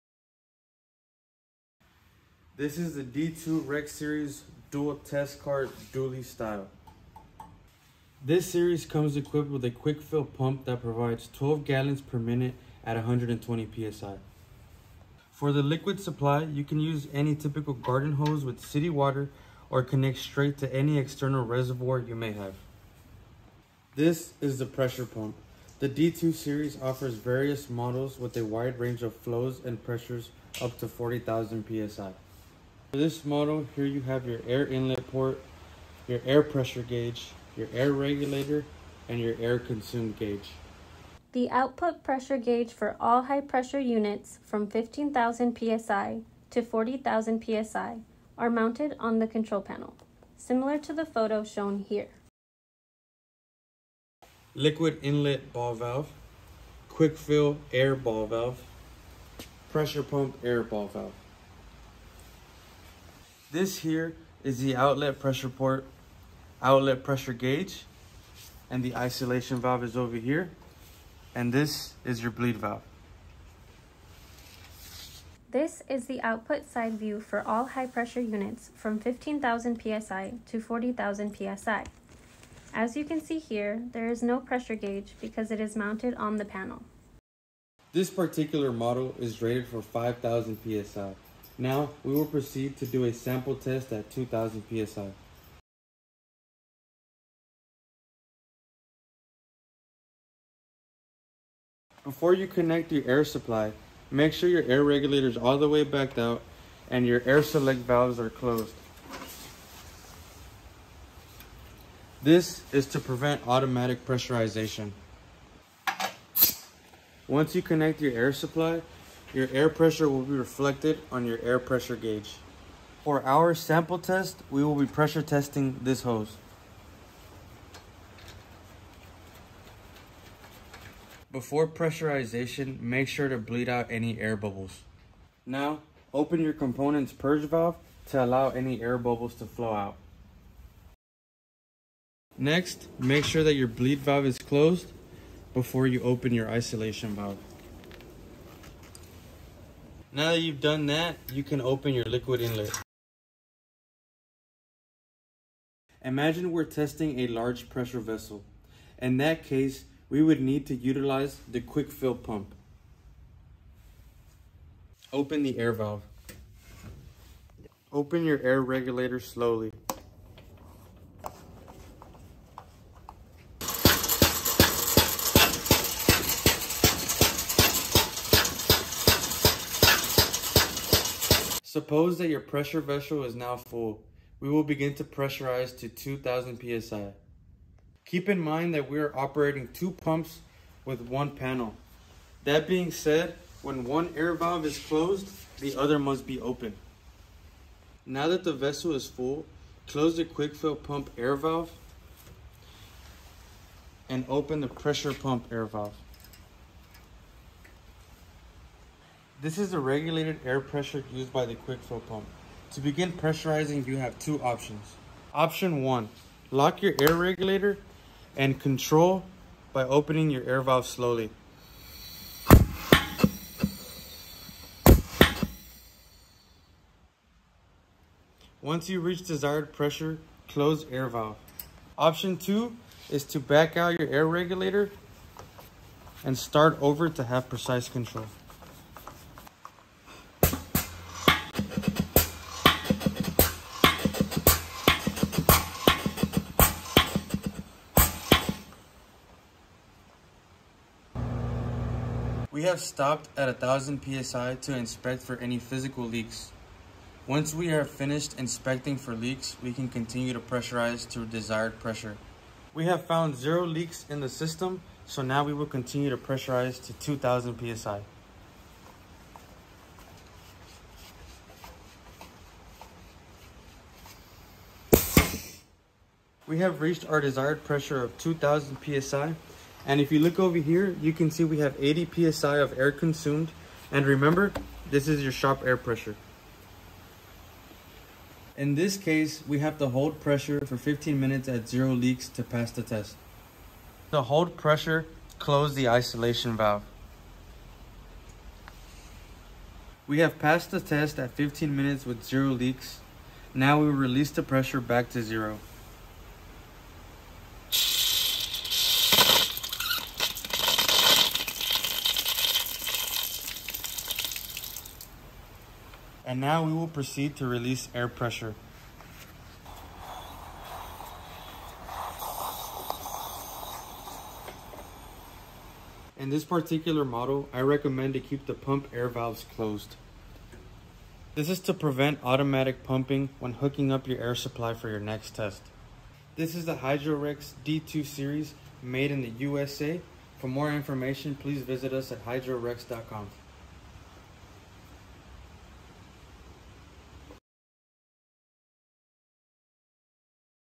Pipe Lines Hydrostatic Testing Pump | Pressure Test Pump for Pipes Spool and Pressure Vessels.
Pipe lines hydrostatic testing pressure test pump for pipes, spools and pressure vessels with the Hydrorex test cart pump dolly style D2-REX dual pressure pumps pressures up to 30000 psi